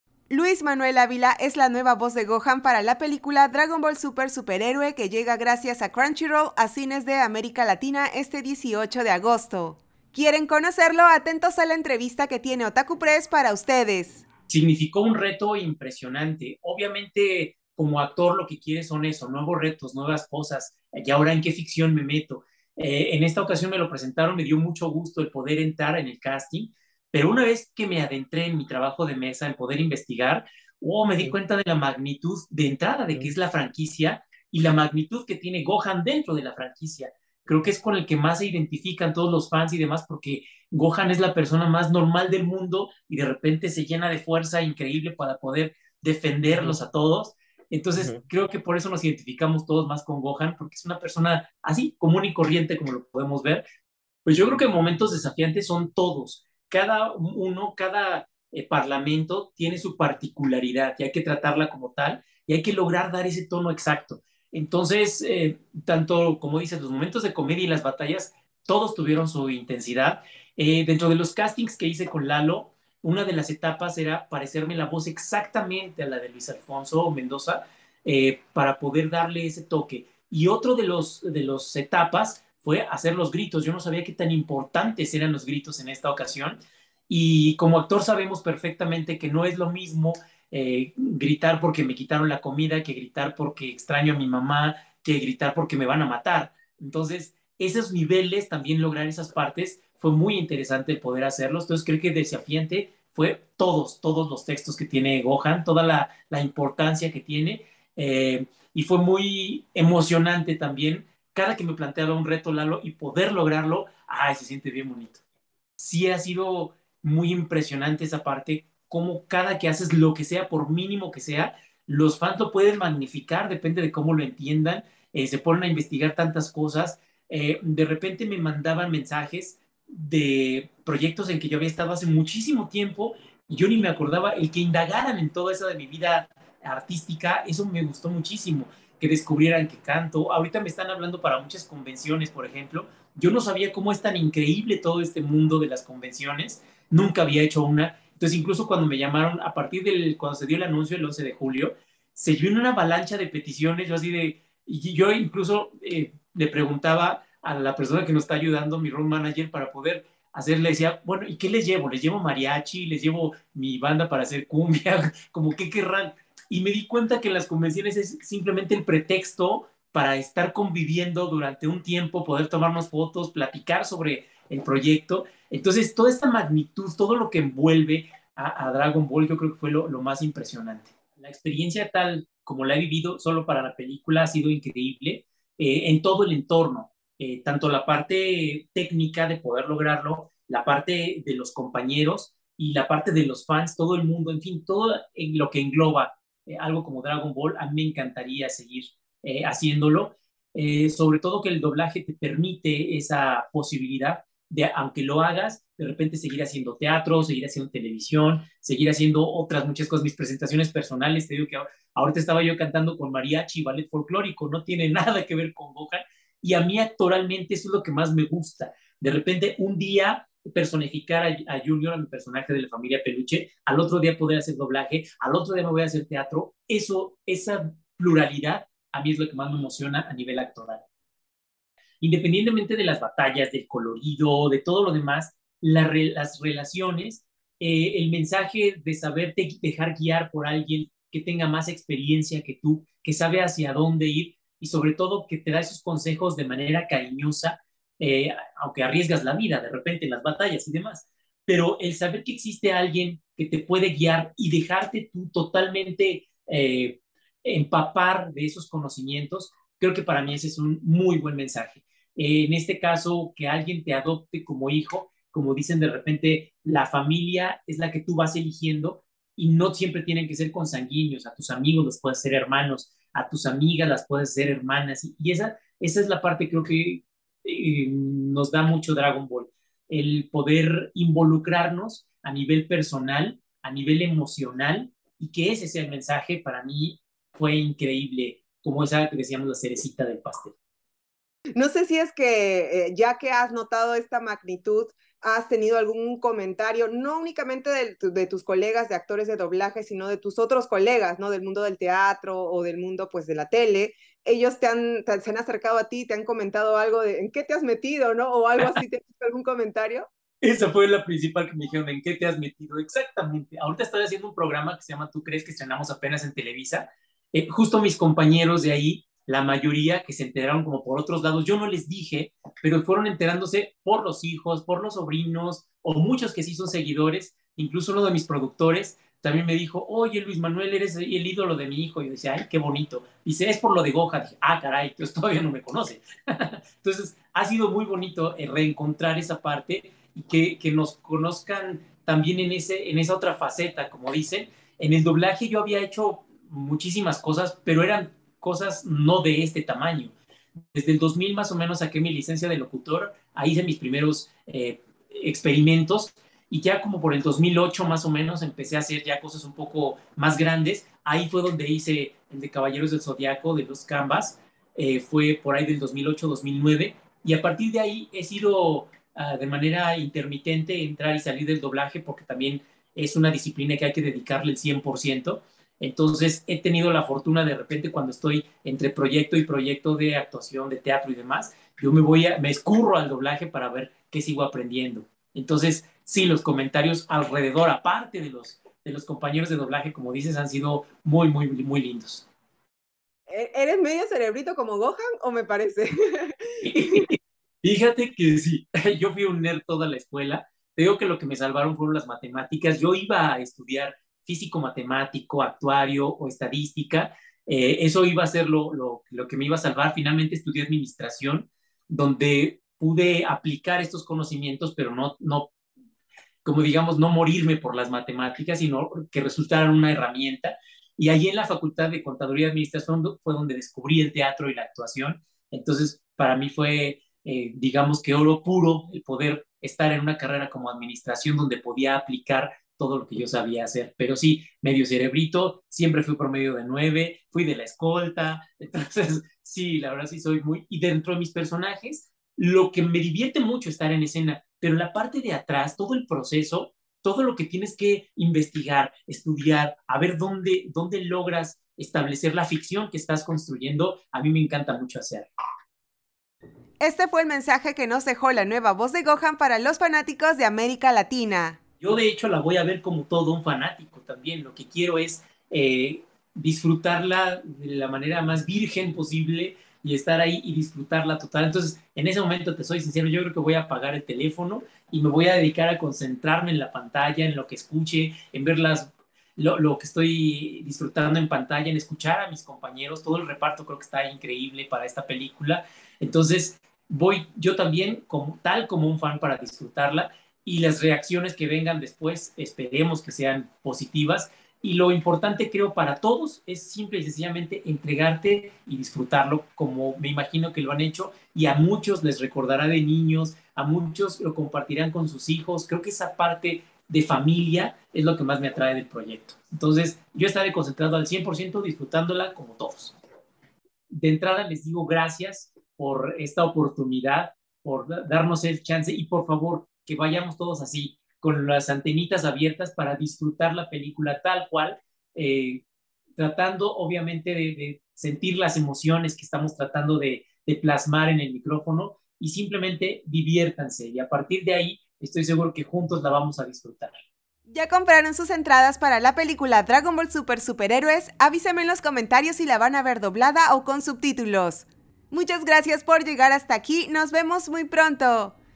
LUIS MANUEL ÁVILA: CADA MOMENTO COMO GOHAN FUE DESAFIANTE | DRAGON BALL SUPER: SUPERHÉROE
El actor Luis Manuel Ávila es la nueva voz de Gohan, y en esta entrevista con Otaku Press contó cómo realizó el doblaje latino para la película Dragon Ball Super: Superhéroe.

La cinta se estrena este 18 de agosto.

En la entrevista vía Zoom también participó @soulidealist gracias a la coordinación de Crunchyroll. Pueden revisar el resto del video desde sus redes 📹

Revisen los videos análisis de los episodios de Hanyo no Yahashime